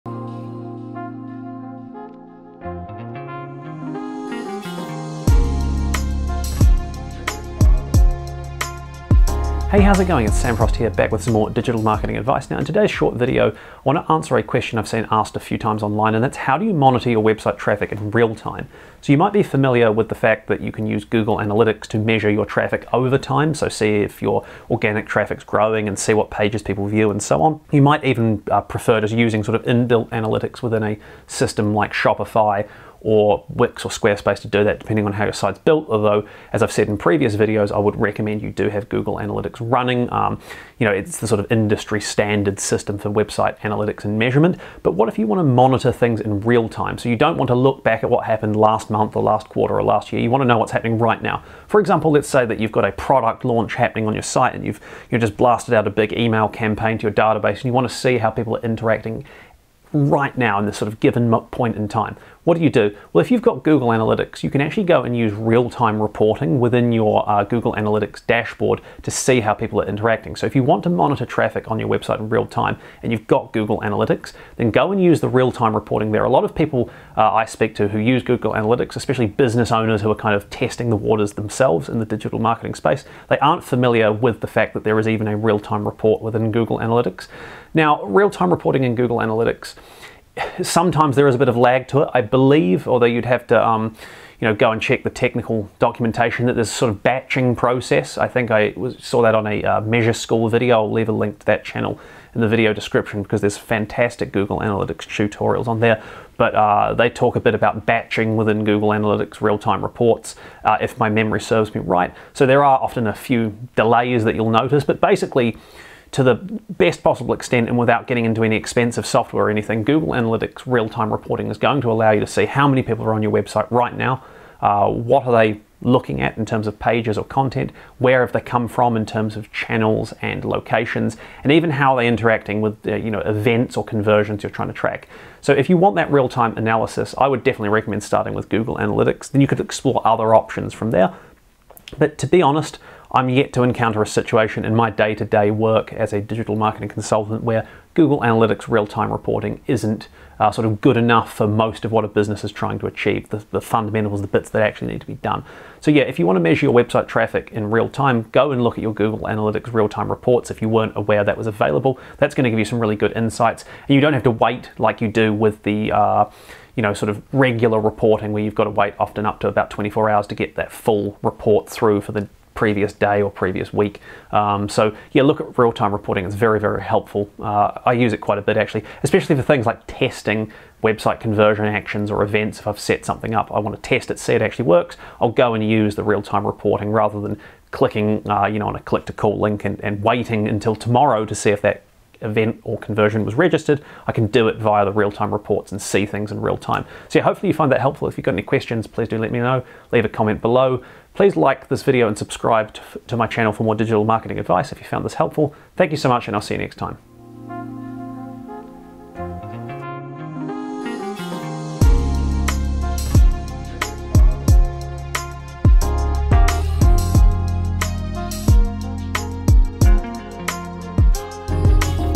How Can You Monitor Website Traffic In Real Time?
It's one thing to monitor your website traffic/visitors "in arrears" - but what if you want to monitor and check in real time?

In this short video I answer the common question "how do I monitor website traffic in real time" and show you just how easy it is to use Google Analytics to check what is happening on your website right now.